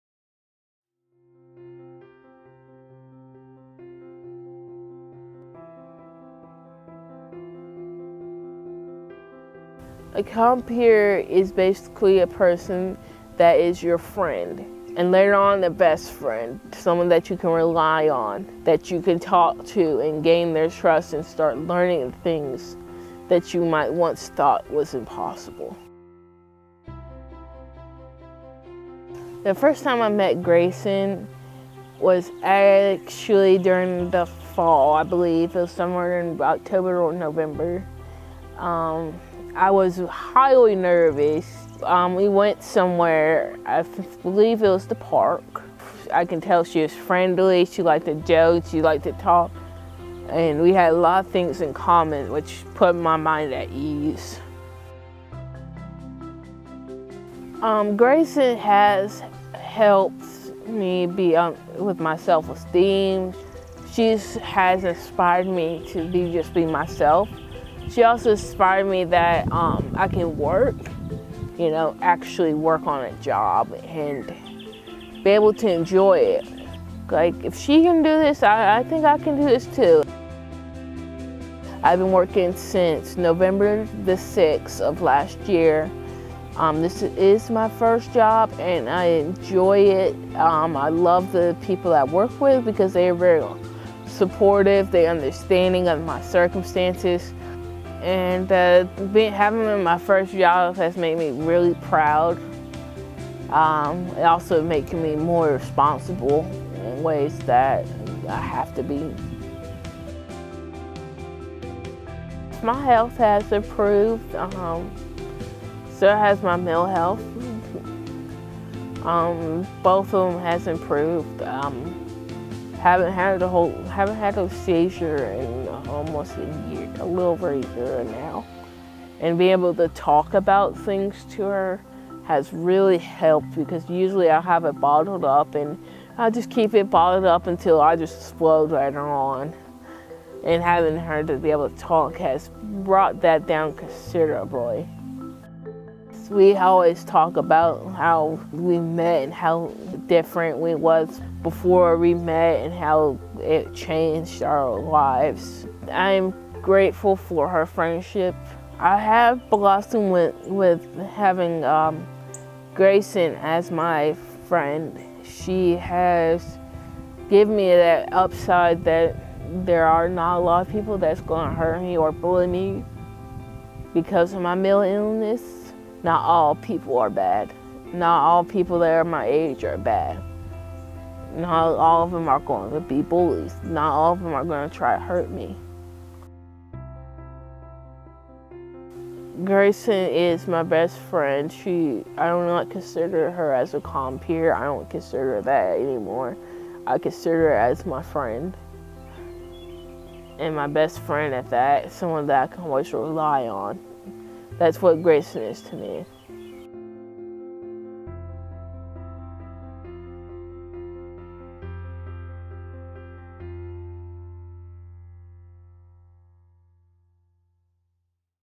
Change a Life. Be a Compeer
MHA Compeer Participant Kyra shares how the program and her friend Gracen have changed her life.

MHA's Compeer is an evidence-based program making friendship matches between compassionate volunteers and adults living with mental health challenges.

MHA has offered Compeer since 1990.

To learn how you can become a compeer visit Mental Health America of Central Carolinas

Compeer matches adults living with a mental health diagnosis with compassionate community volunteers who provide one-to-one supportive friendship and mentoring relationships to offset the social isolation and loneliness that can accompany mental illness.

Compeer matches meet virtually or face-to-face at least twice a month. Many go to a movie, have lunch, walk and talk, or enjoy other social activities together. Compeer volunteers make a minumum one year commitment to the friendship.